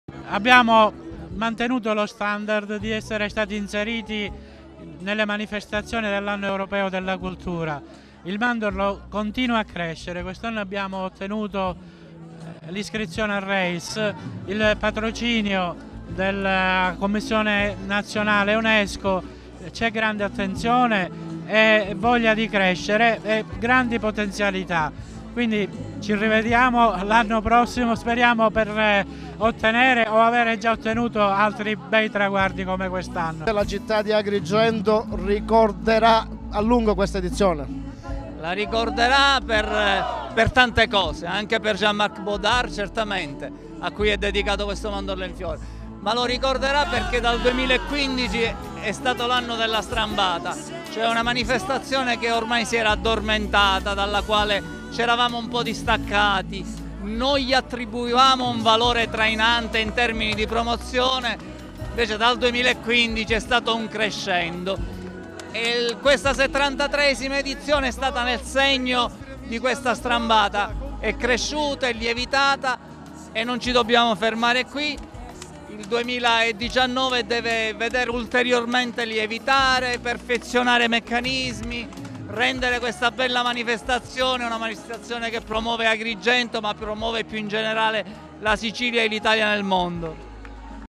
Mandorlo 2018: Tempio d'oro alla Georgia
Alla Georgia il Tempio d’Oro della 73° edizione della Festa del Mandorlo in Fiore. Poi Argentina e Armenia. I vincitori sono frutto, per la prima volta, del voto popolare.